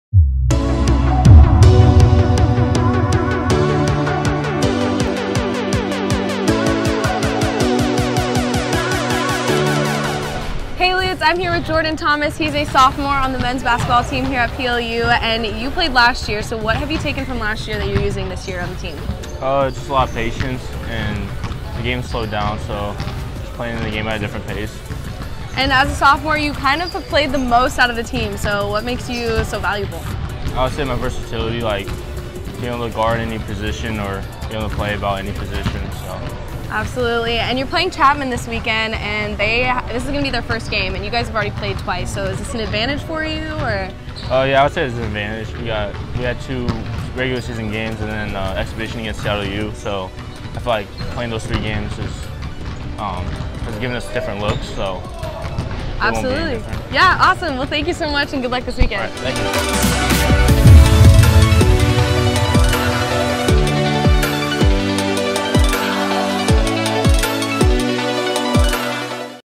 PLU MBB Jordan Thomas 11/21/19
Pacific Lutheran University sophomore men's basketball player Jordan Thomas talked with classmate Hannah Wisti about this weekend's upcoming tournament at the University of Puget Sound.